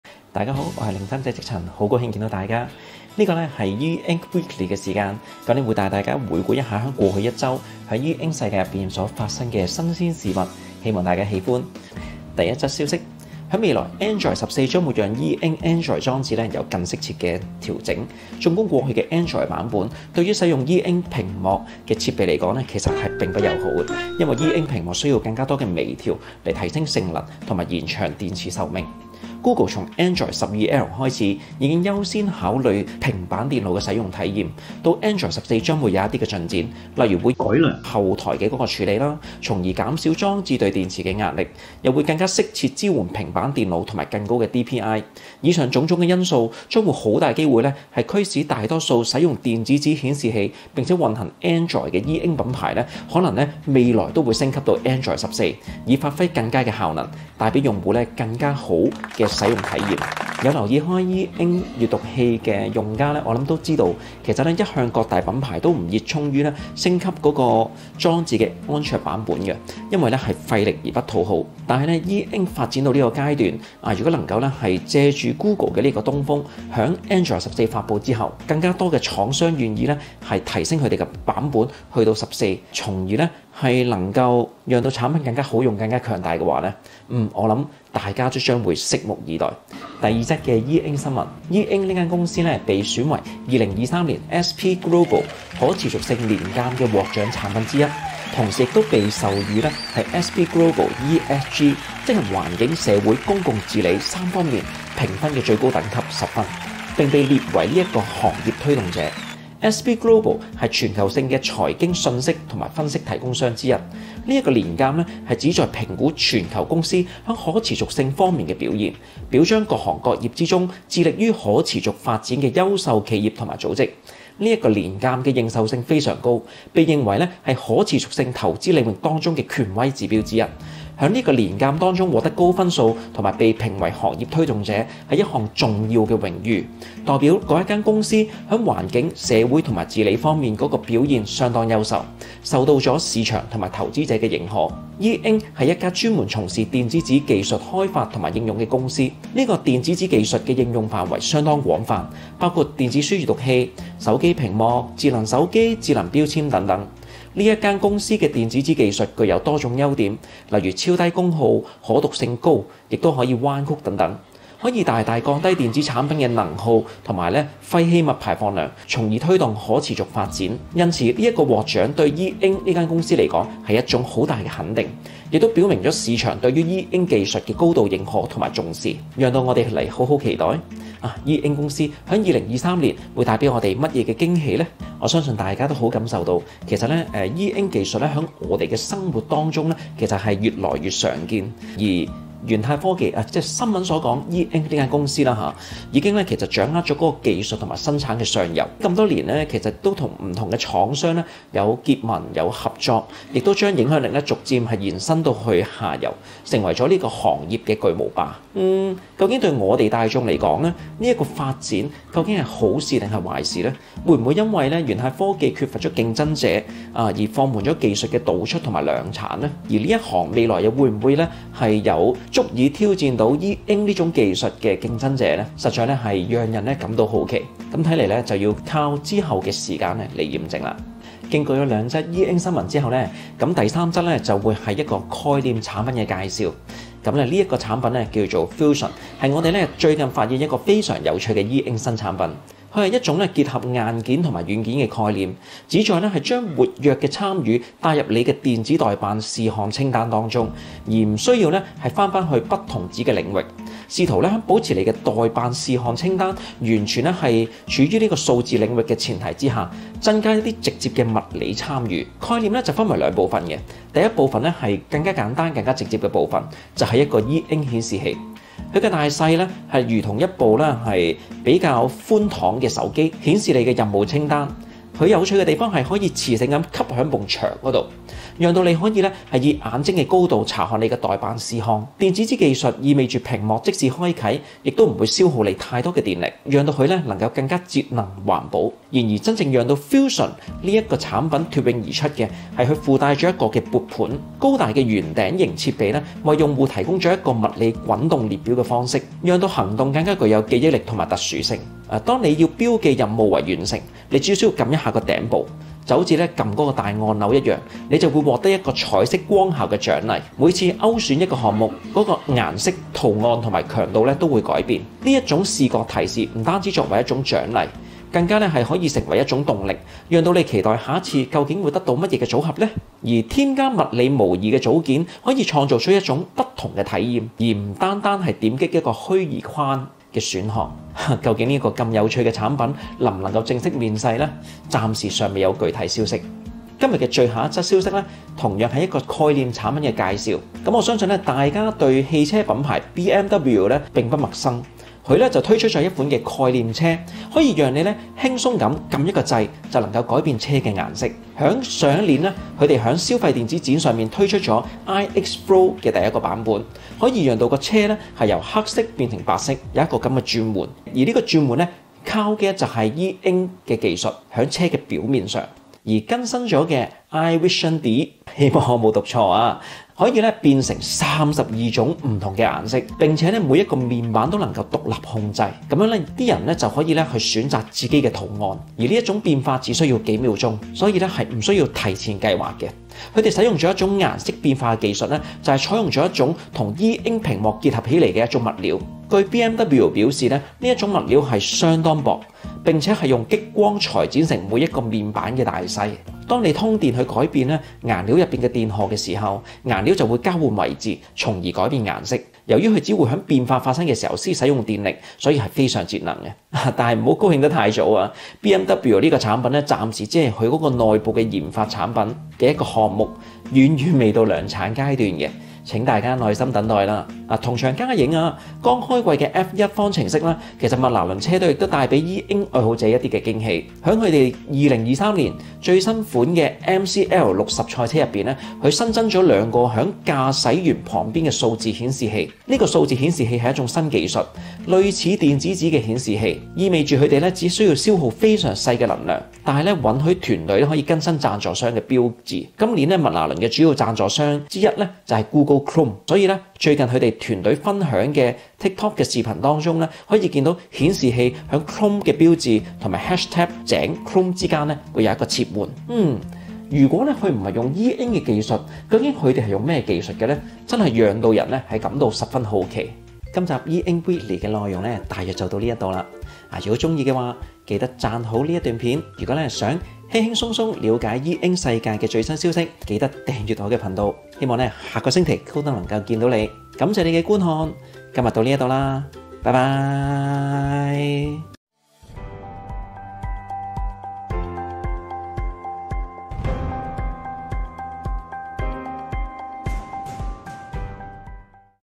Android14的期待、E-Ink公司獲獎、概念產品Fusion、BMW i Vision DEE【E-INK WEEKLY 23年3月#001】（with English Subtitles）
（CC English Subtitles）
若果你想輕鬆了解E-Ink世界的新資訊，記得留意【E-INK WEEKLY 】!

本周E-Ink新聞如下：
00:12 E-Ink新聞：請期待未來的E-Ink閱讀器/平板邁進Android 14的世界吧！
01:30 E-Ink新聞：E-Ink獲得2023年度S&P Global可持續性年鑑認證
03:51 概念產品介紹：Fusion──E Ink牆面顯示屏和桌面轉盤
05:37 概念產品介紹：BMW i Vision Dee──一鍵化身變色龍

1. E-Ink News: Please look forward to the future E-Ink Reader/Tablet entering the world of Android 14!
2. E-Ink News: E-Ink Receives 2023 S&P Global Sustainability Yearbook Certification
3. Concept Product Introduction: Fusion──E Ink Wall Display and Desktop Revolving Stand
4. Concept Product Introduction: BMW i Vision Dee──Transforms with the Push of a Button, Like a Chameleon.

若你或你的親友人在英國，需要購買電子閱讀器的話，不妨去我經營的網店逛逛：
若你願意支持我繼續製作影片，歡迎buy me a coffee；想學習硬筆書法的，可成為我的月費會員，感謝。